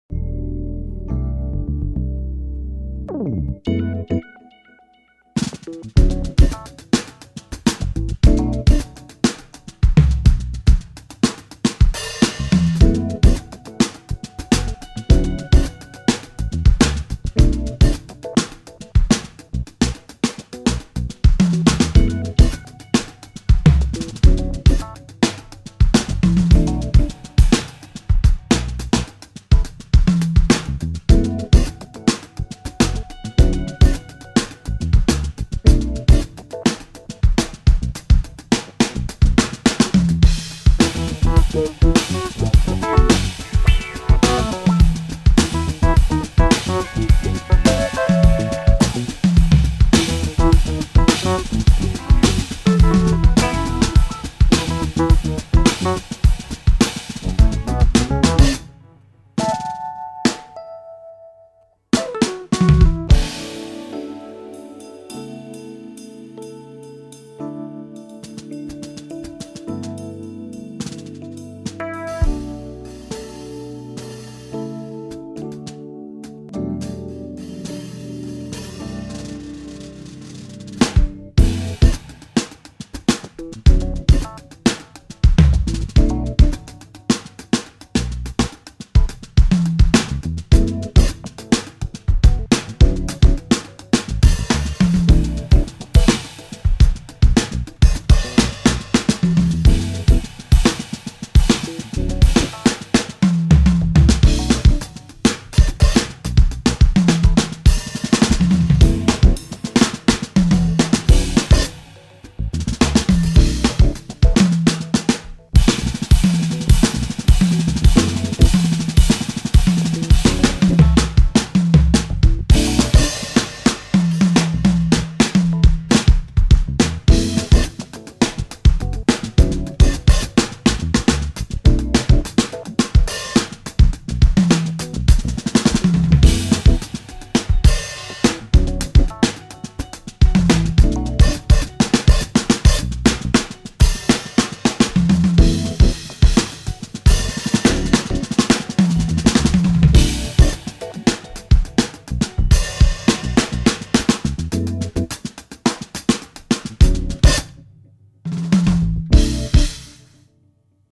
Another day another jam.
Blowing over another Jeff Lorber idea. This one will be included in my play along album. Audio, with and without drums and video available in my app. All transcriptions will be available as a digital booklet.